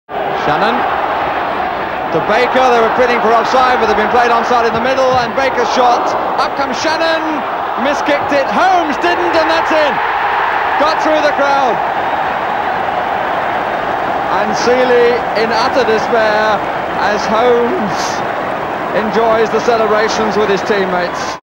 Southampton v Coventry 1980/81
The ever-dependable Nick Holmes gets the only goal at The Dell against Coventry.